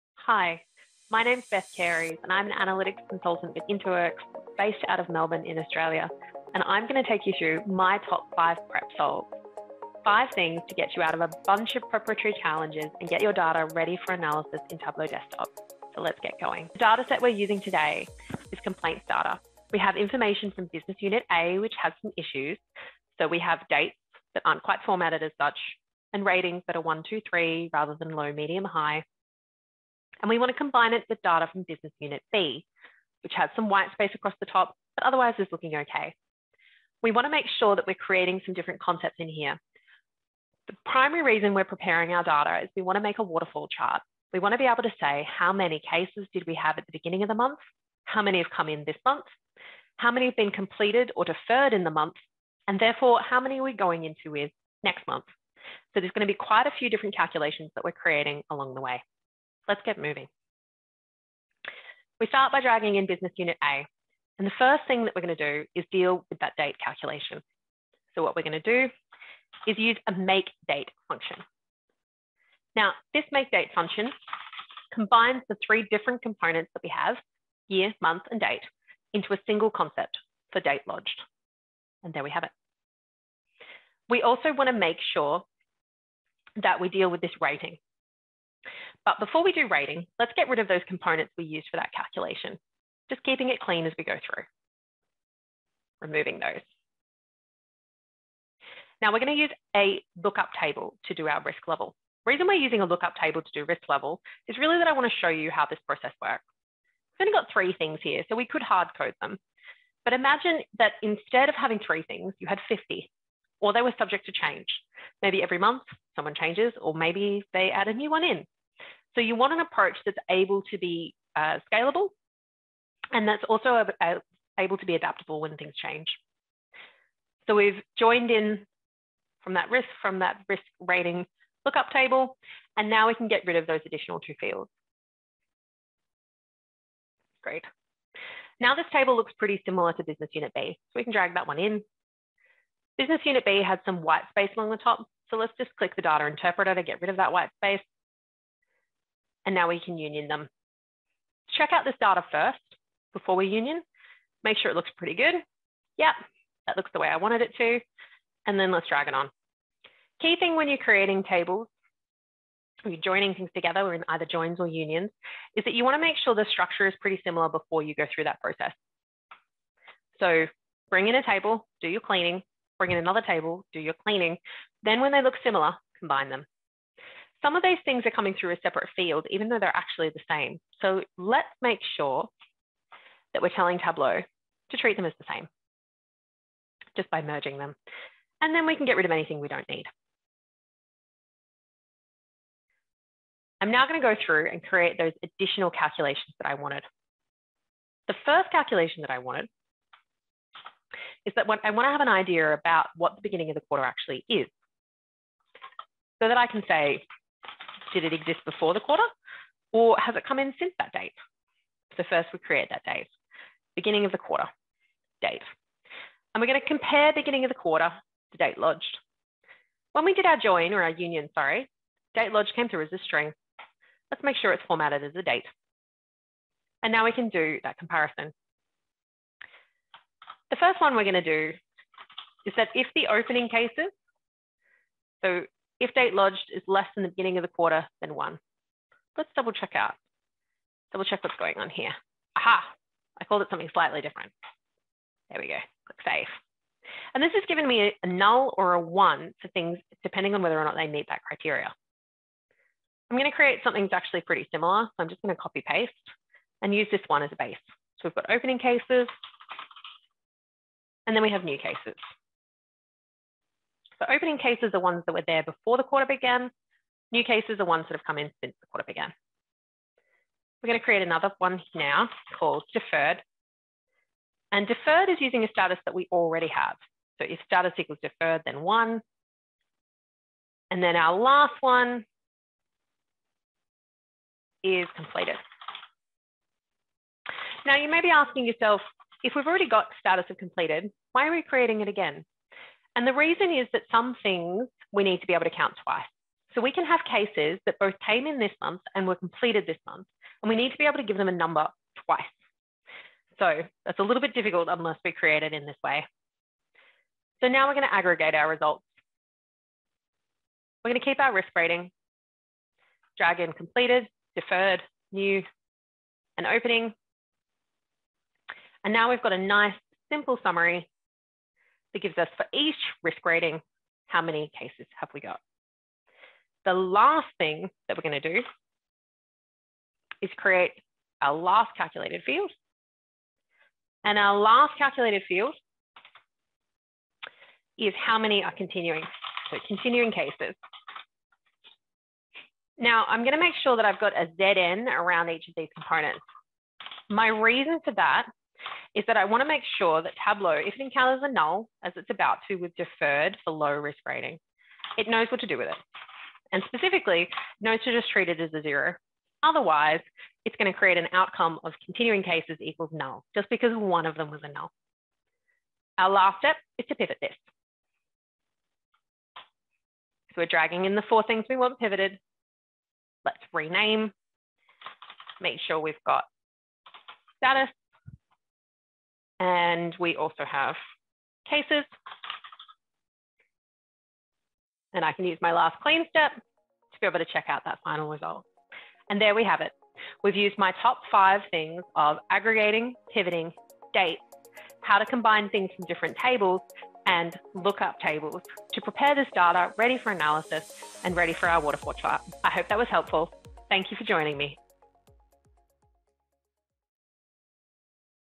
My Top Five Tableau Prep Solutions
Beth shares her top five go-to moves in Tableau Prep in this example of creating a waterfall chart.